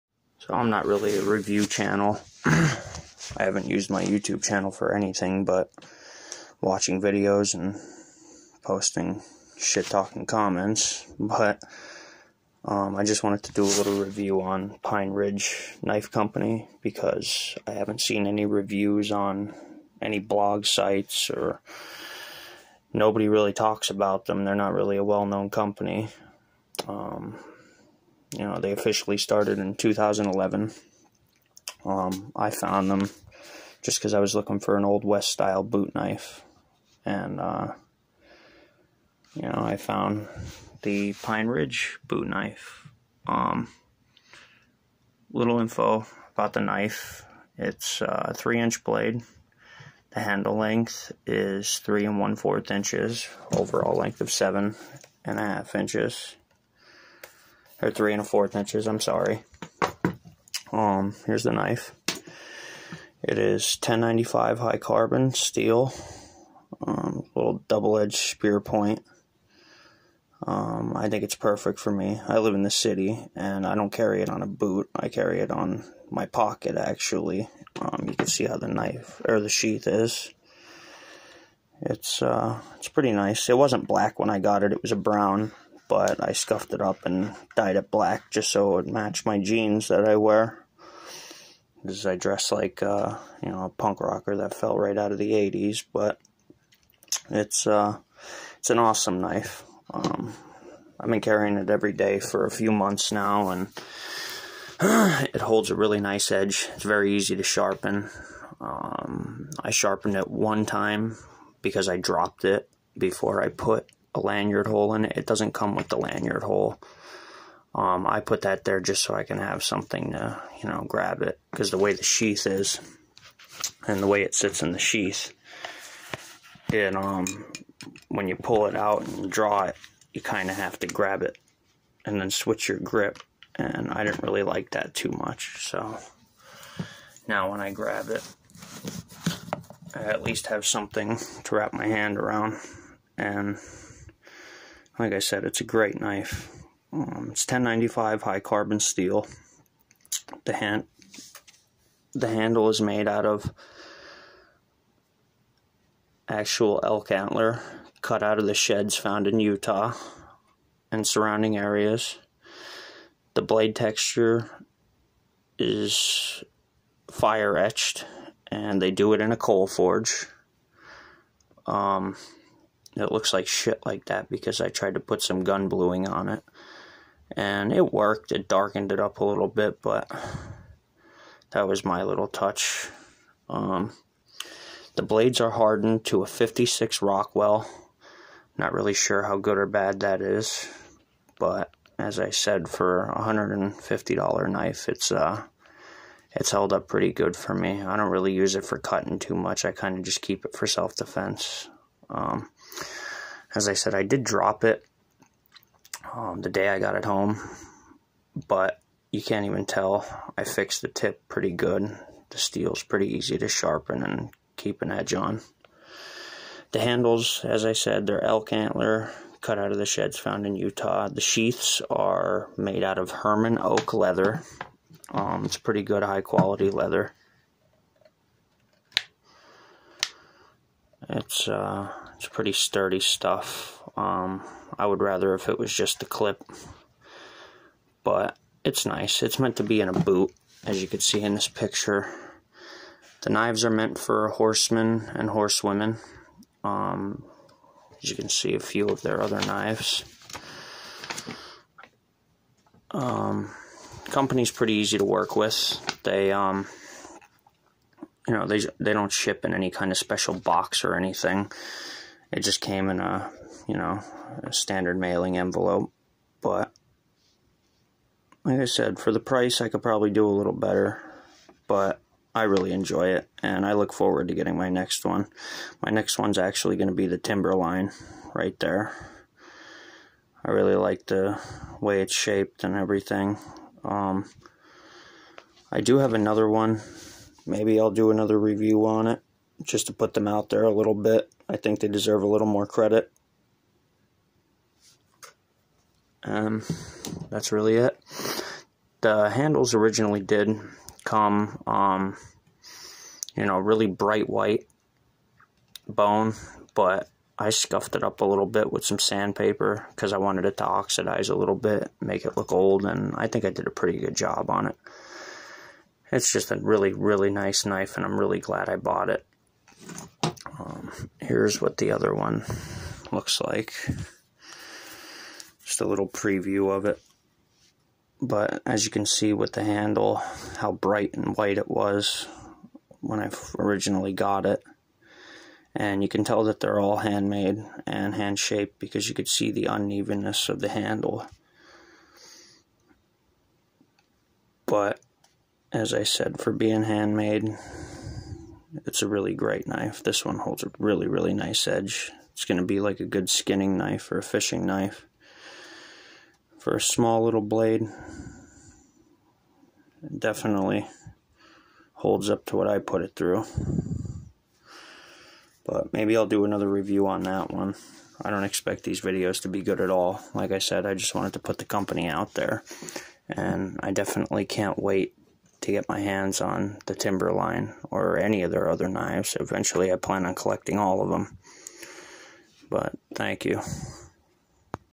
Pine ridge boot knife
Haven't seen any reviews on blog websites or YouTube about the pine ridge company at all just wanted to show them a little love because I really enjoy the knives that I got really great company and quality knife